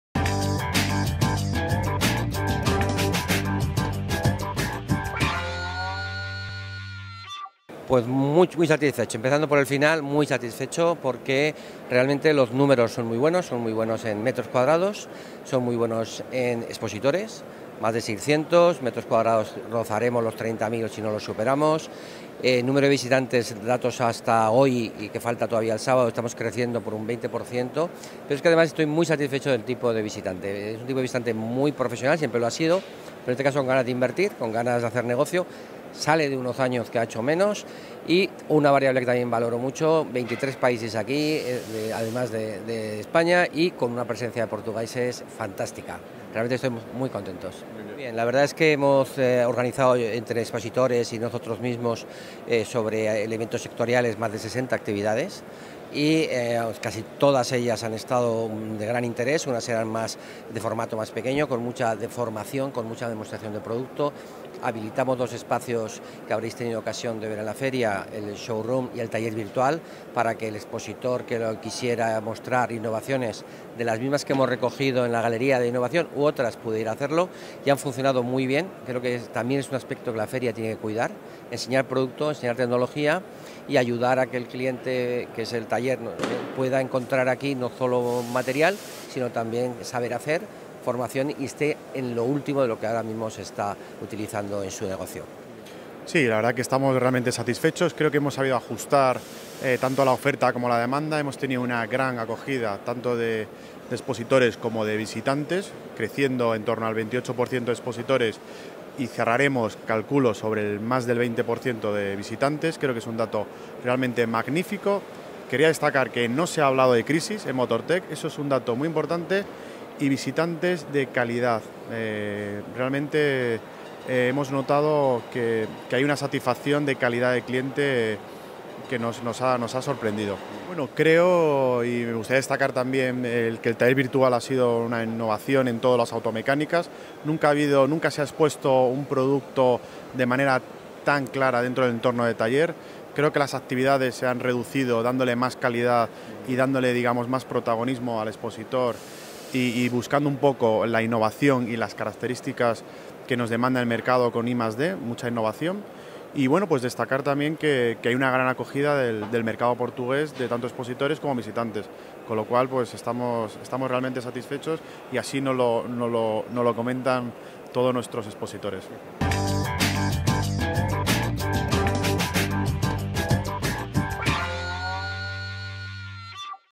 2015 - Motortec - Ifema Motortec Automechanika Madrid
Entrevista a Miguel Aguilar, Diretor Motortec e Pedro Pedrero, Diretor Comercial Motortec, sobre o balanço da edição Motortec Automechanika Madrid 2015